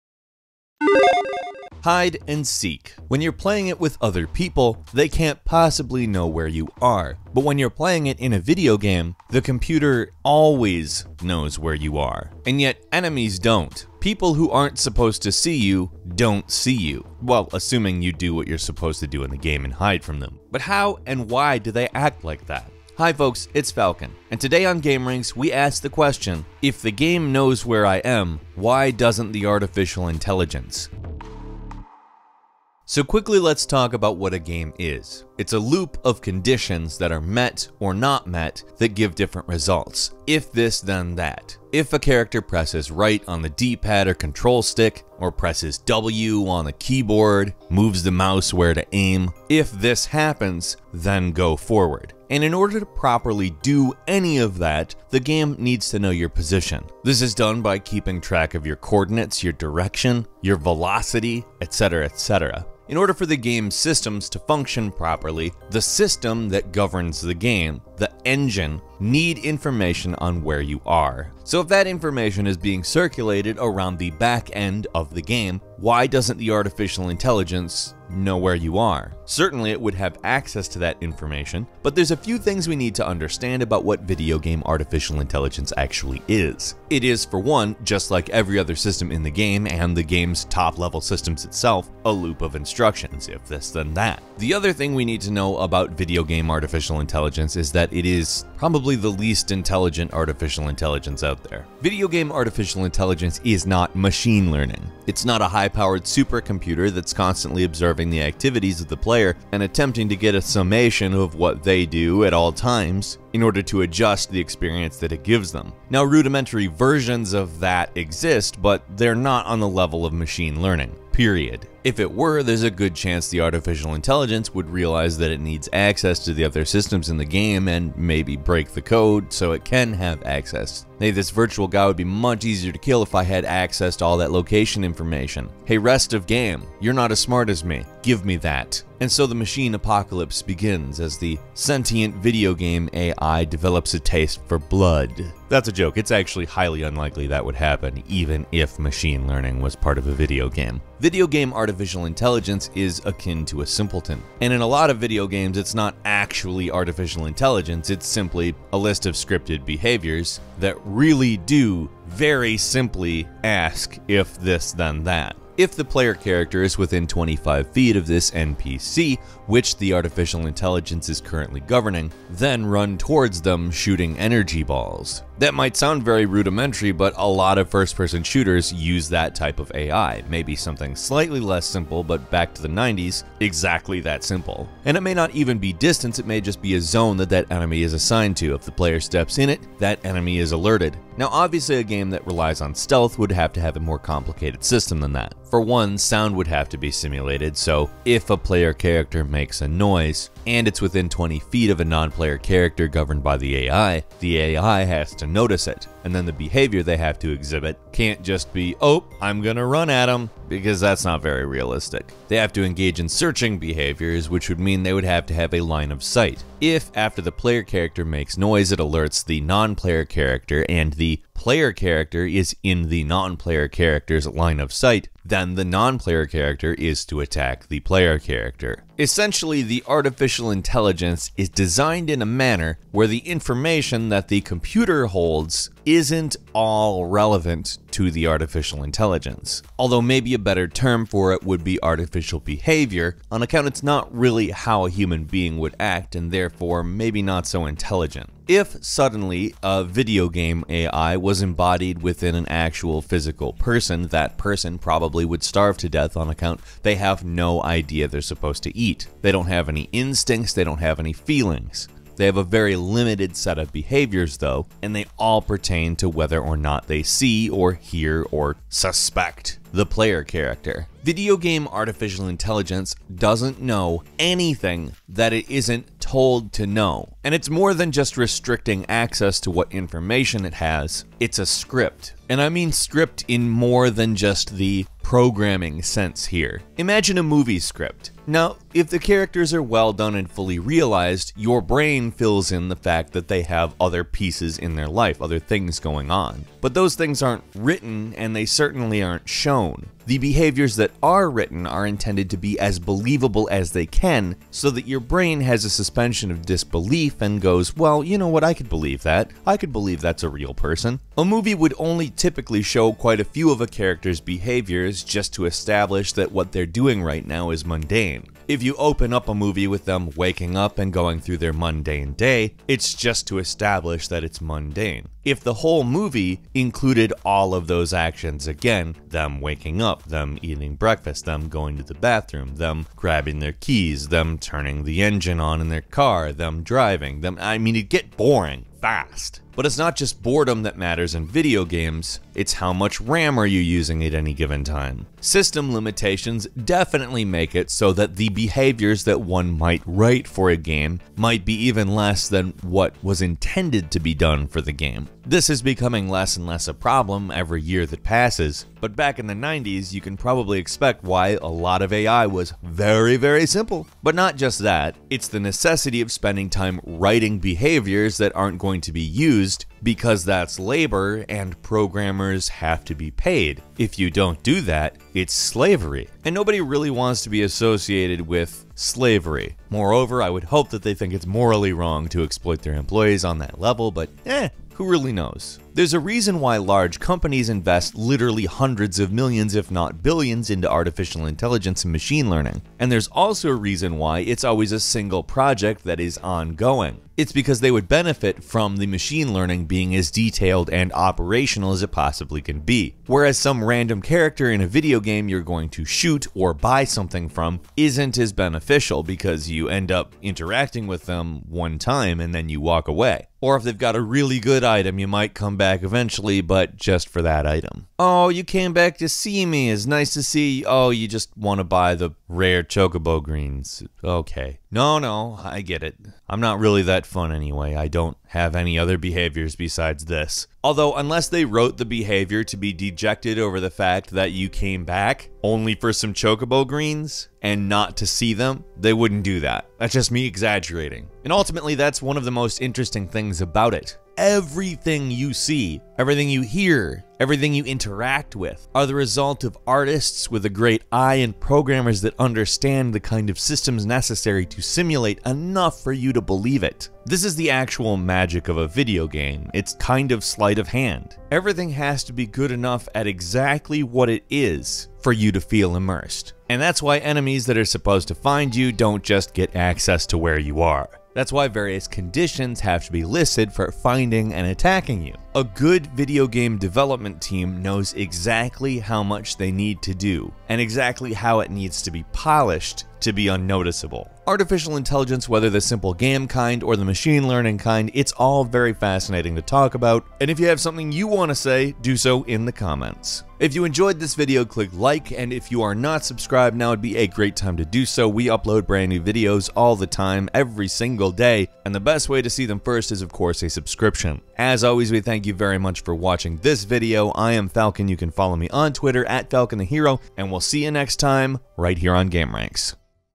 How Games Use Artificial Intelligence To Play Hide & Seek
We've always had AI systems in games, but how exactly do things work when an enemy searches for you while the computer always knows where you are in the first place? Let's talk.